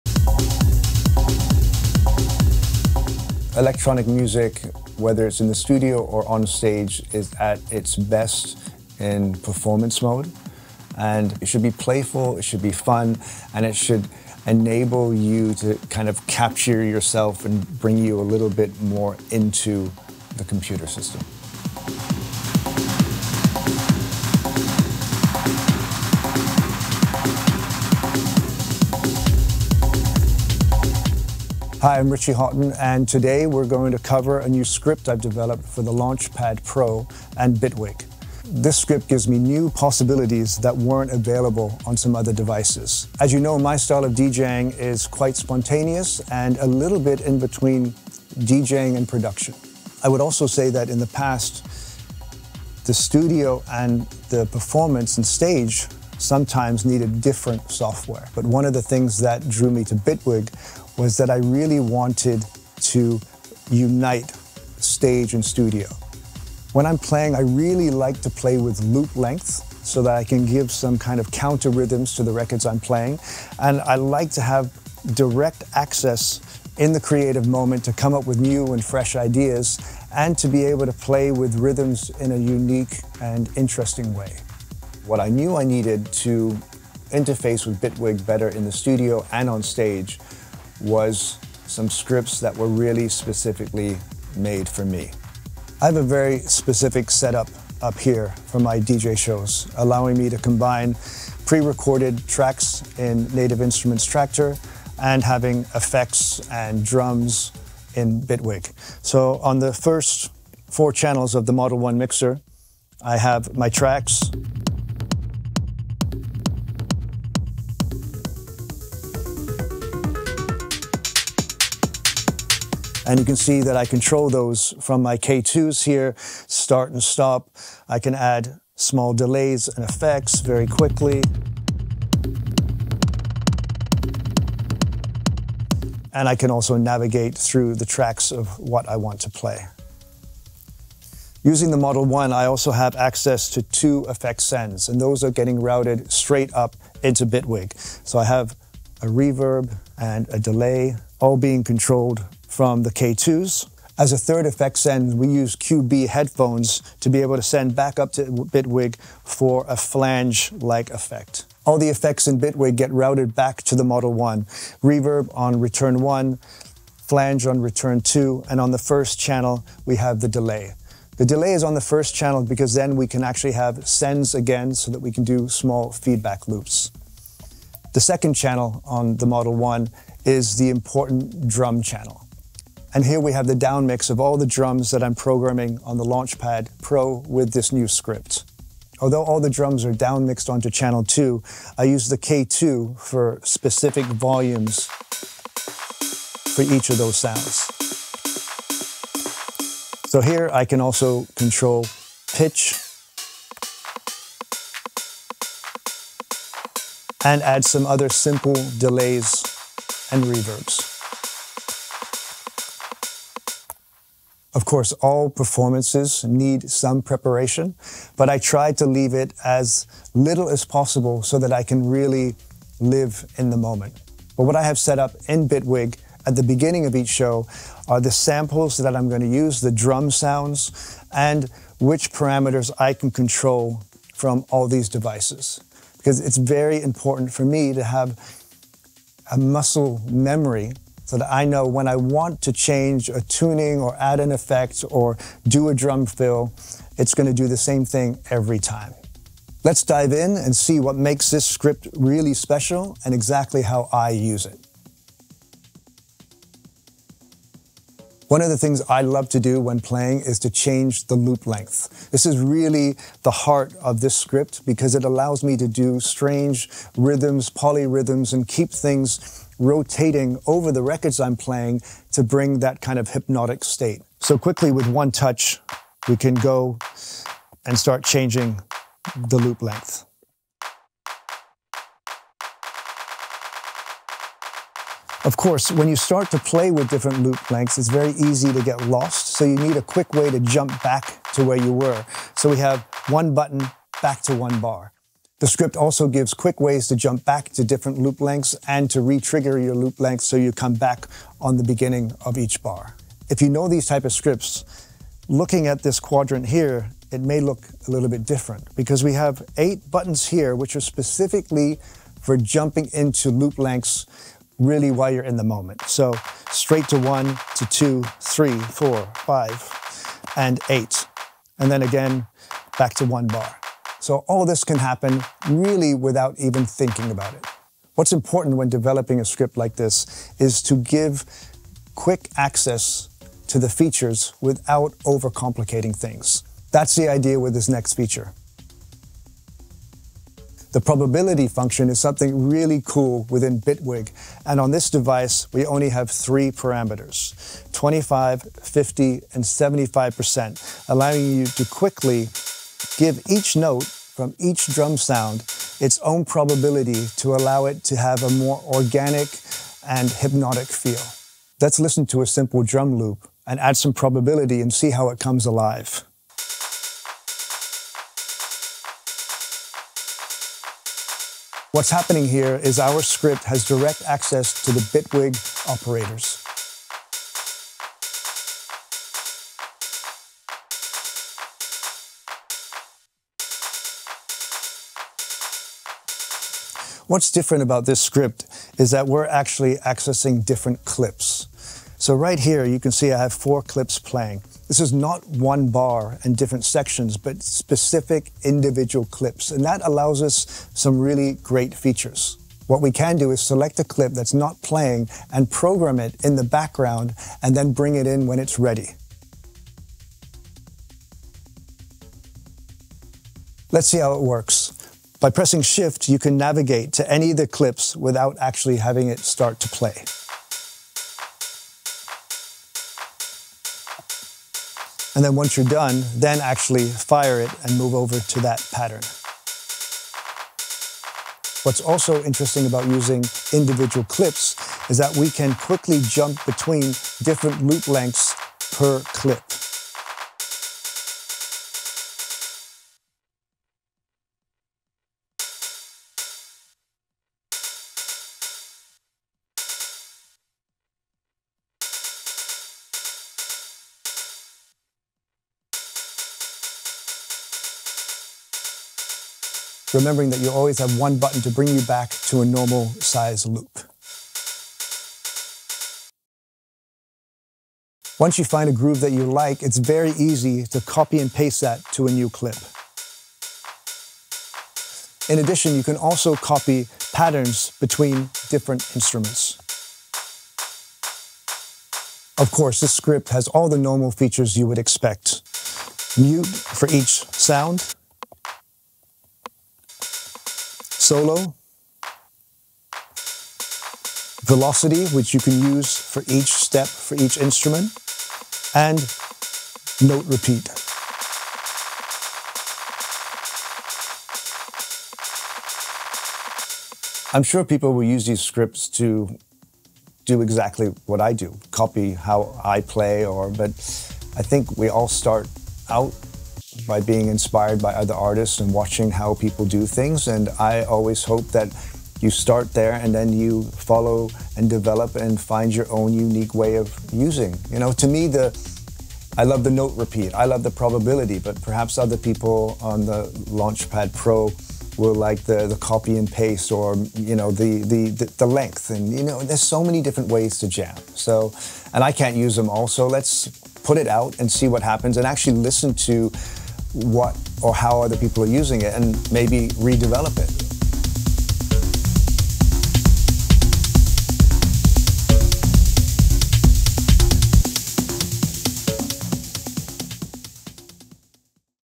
Richie Hawtin's Live Setup Using Bitwig Studio
Richie Hawtin explains his Bitwig Studio live performance setup using Traktor virtual DJ software and custom MIDI controller scripts. In this video he shows how his script for the Novation Launchpad Pro controls Bitwig note clips and Operators for drums and melodies.

Contents:
0:00 Intro
1:36 Setup overview
3:02 K2 controls Bitwig Studio parameters
4:16 Loop length function
5:38 Probability function
7:47 Copy and paste function
8:04 Mute, solo, velocity & note repeat